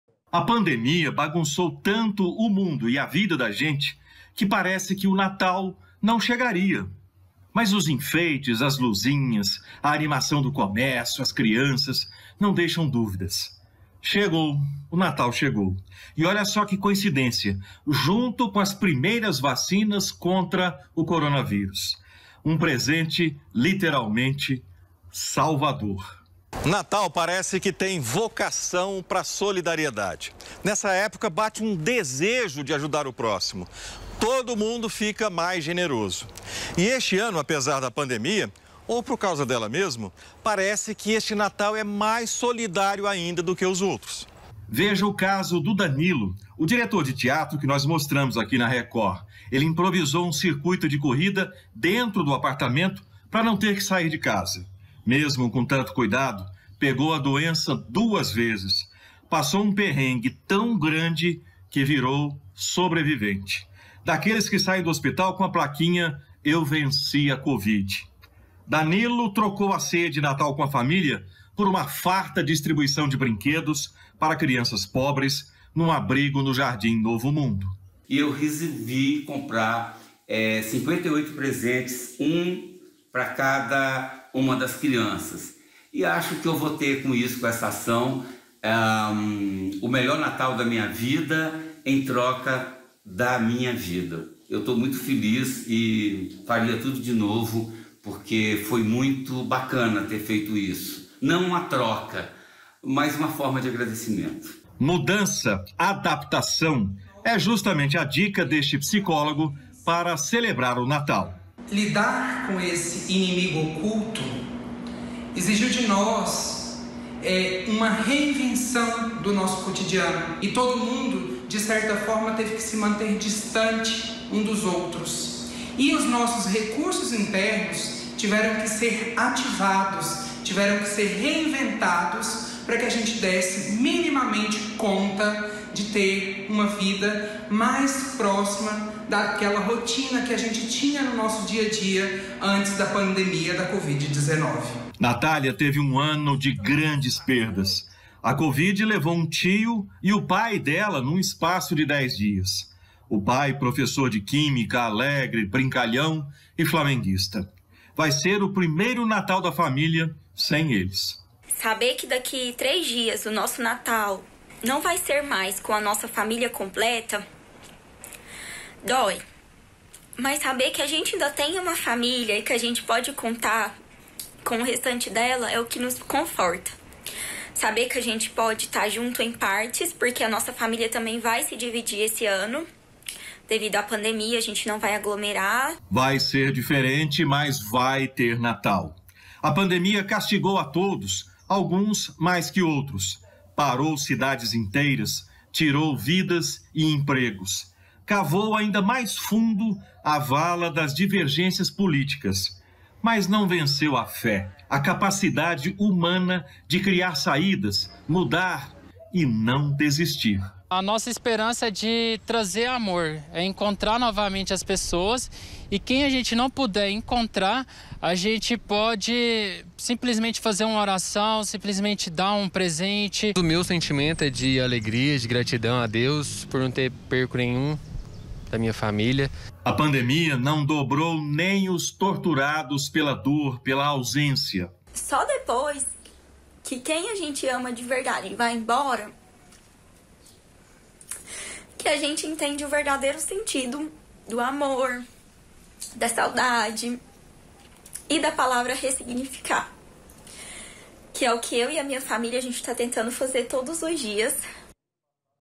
NATAL EM TEMPO DE PANDEMIA: ÉPOCA DE ESQUECER AS DORES E FESTEJAR A VIDA
ESTAMOS HÁ TRÊS DIAS DO NATAL DE UM ANO DESAFIADOR. MUITA GENTE PASSOU POR PERDAS IRREPARÁVEIS EM 2020. MAS MESMO ASSIM, VÃO PRECISAR RESSIGNIFICAR AS DORES PARA COMEMORAR UM CERTO RENASCIMENTO. CADA UM A SEU MODO, MAS COM ALEGRIA E GRATIDÃO PELA VIDA.